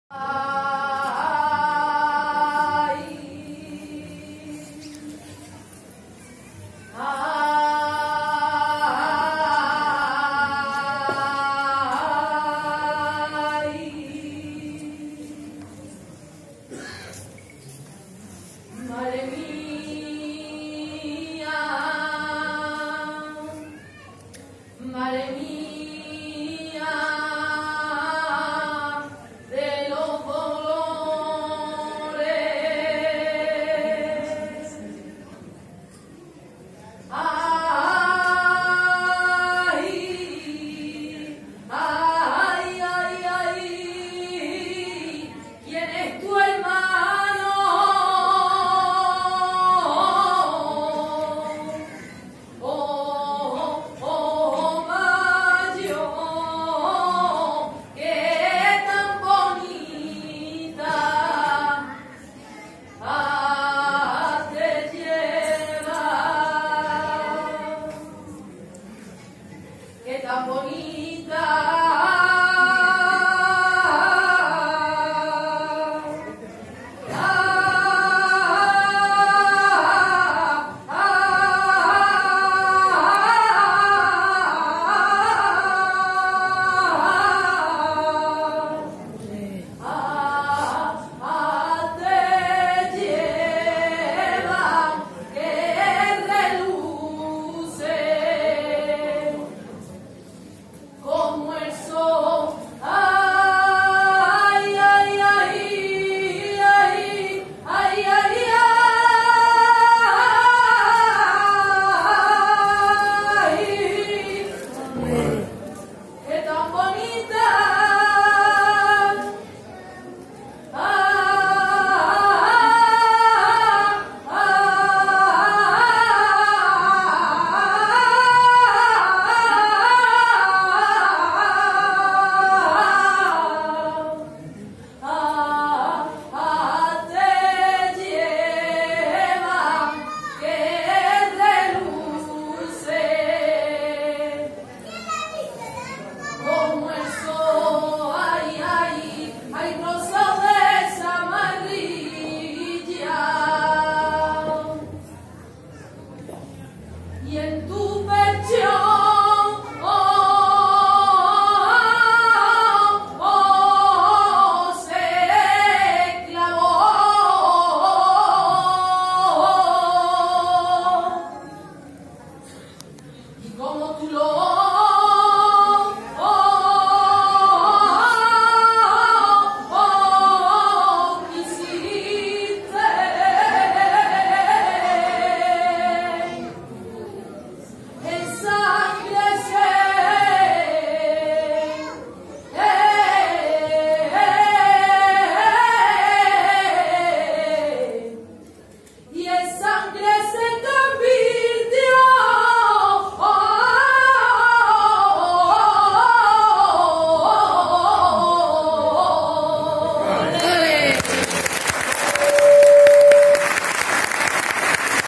BELEN SERRANO " SAETA" GENIAL..
Dia 12 DE Abril  Viernes...Lugar Castro del Río..Concurso de Saetas..Actuación  en directo se la gran Belén Serrano, pero que grande eres y que bonito lo haces todo guapísima  olé tú. 🤩👍👏...